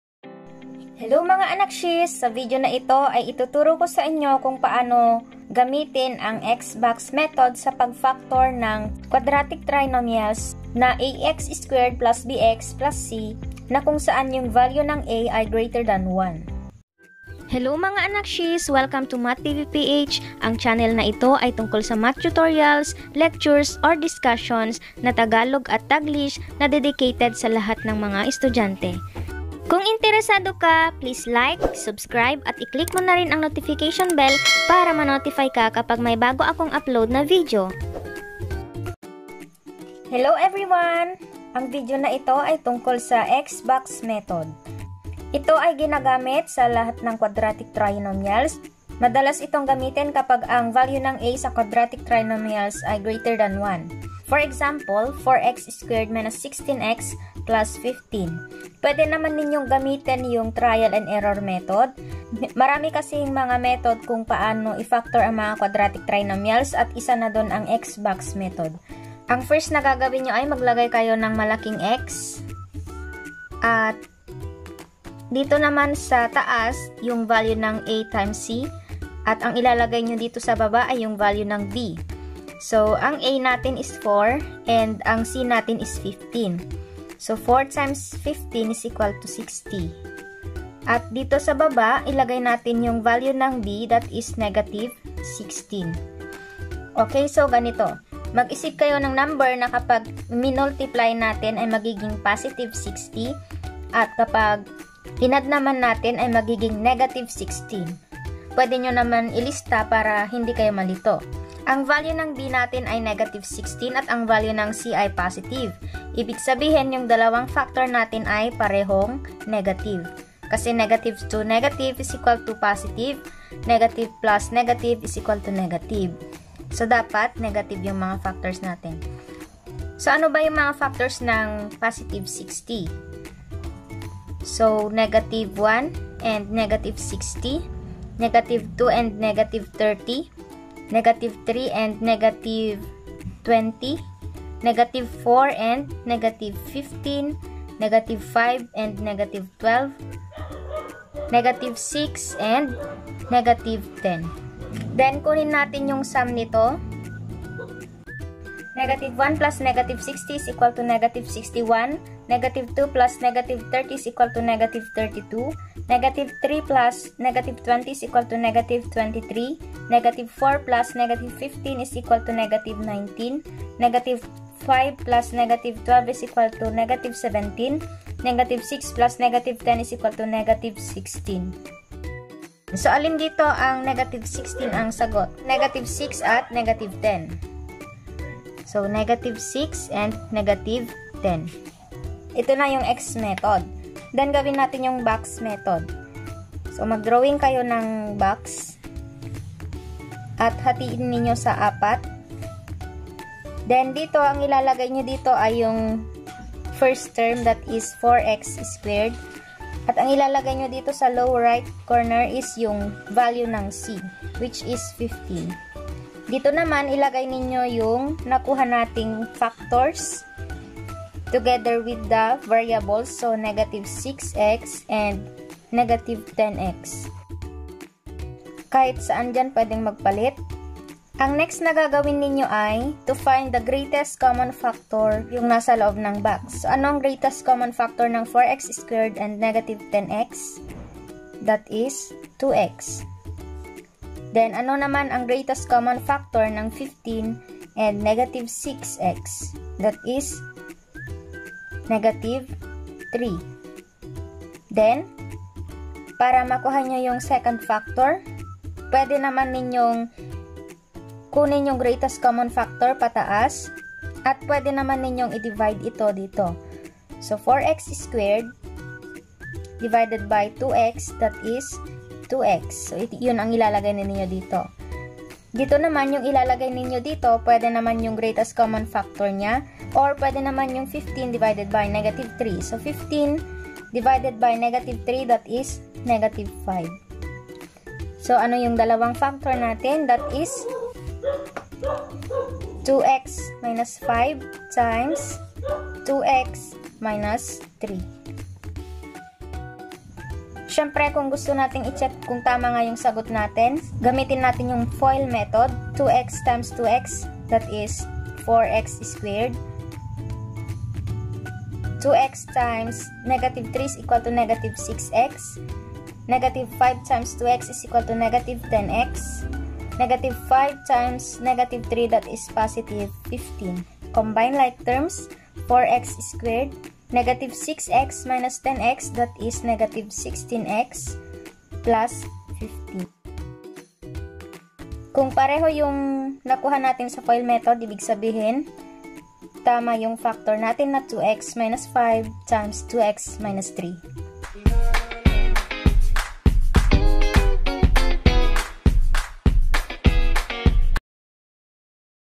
Factoring ax²+bx+c using the Xbox Method | Tagalog Tutorial | MathTV PH
Ang video na ito ay tungkol sa factoring quadratic trinomials gamit ang xbox method.